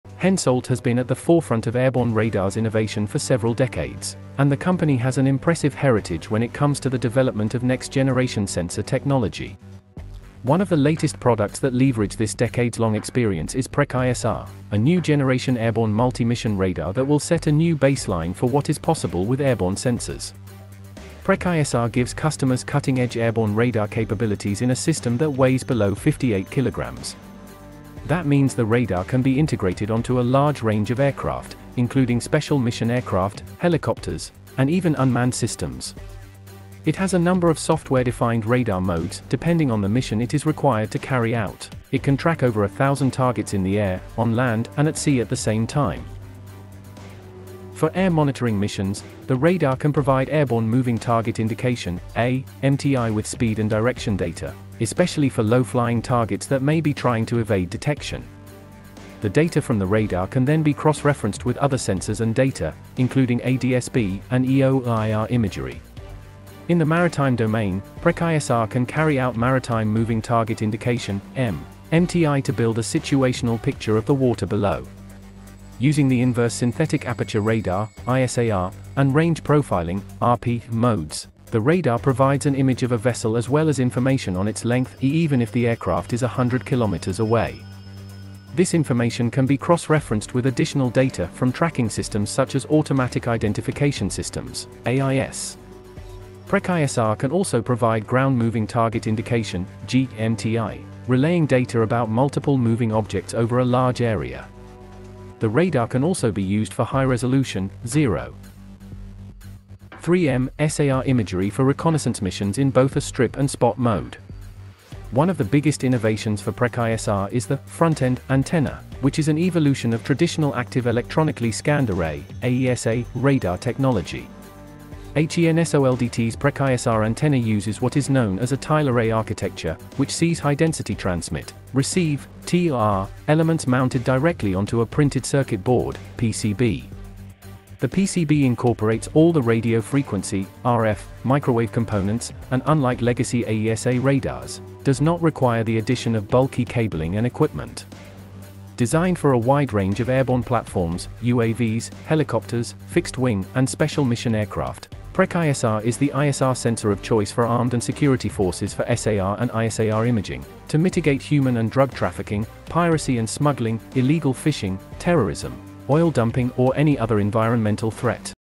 HENSOLDT innovates with its PrecISR powerful radar for airborne ISR platforms
HENSOLDT has been at the forefront of airborne radars innovation for several decades and the company has an impressive heritage when it comes to the development of next-generation sensor technology. One of the latest products that leverage this decades-long experience is PrecISR, a new generation airborne multi-mission radar that will set a new baseline for what is possible with airborne sensors.